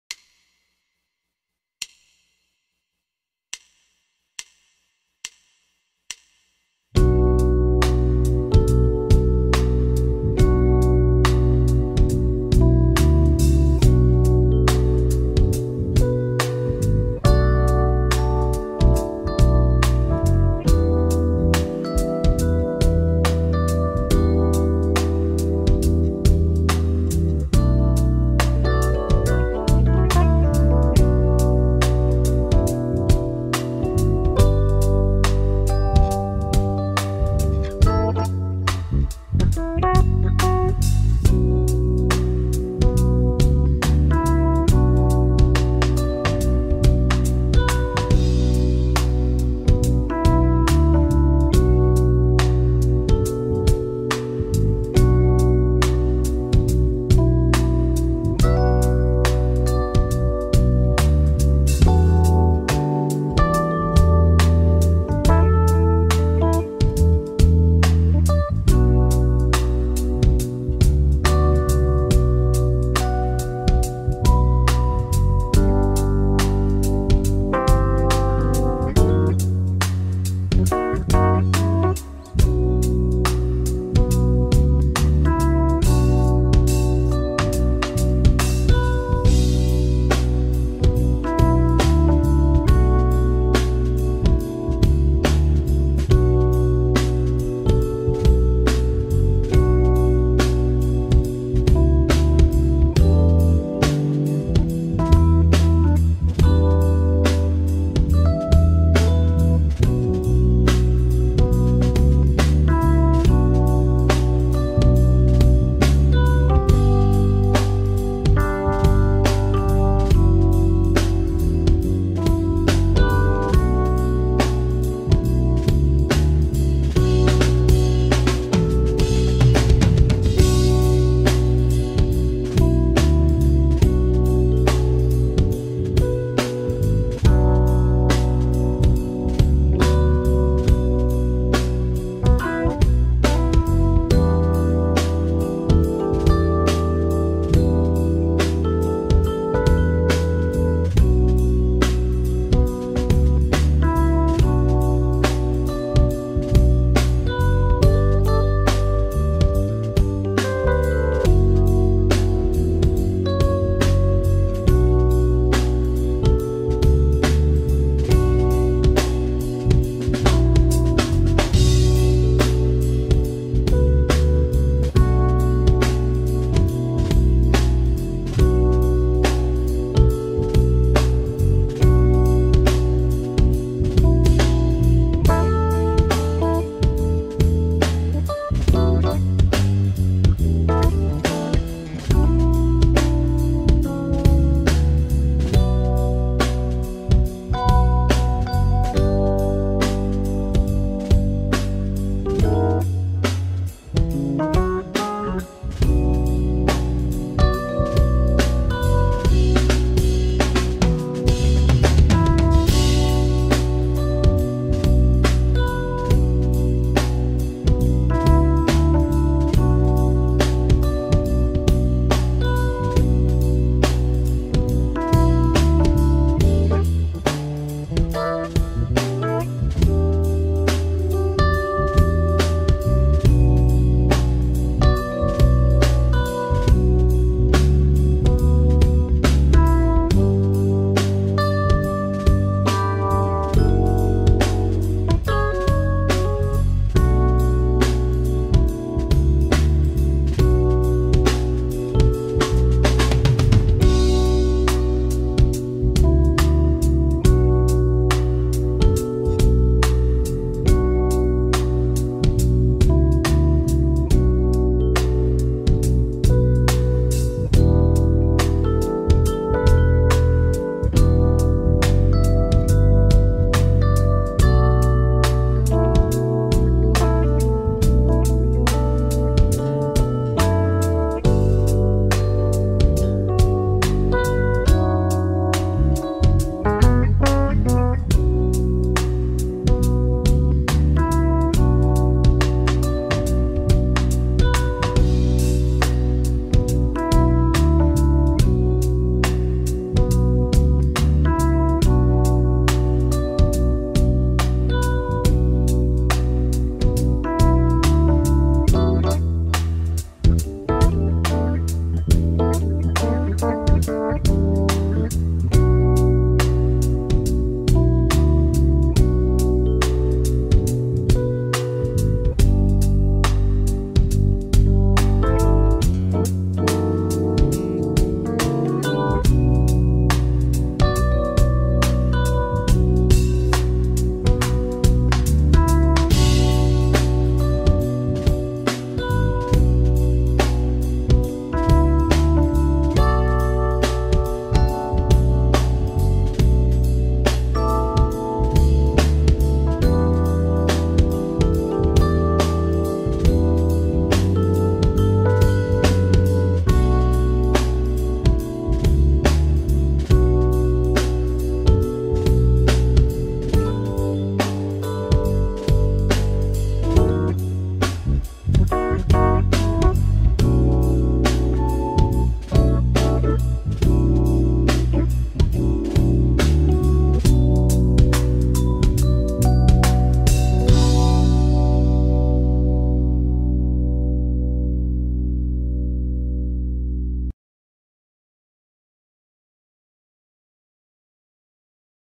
Slow Blues Minor Bm Backing Track
slow Blues in key B minor. progression Bmx4bar - Em7x2bar - Bmx2bar - G - F -Bmx2bar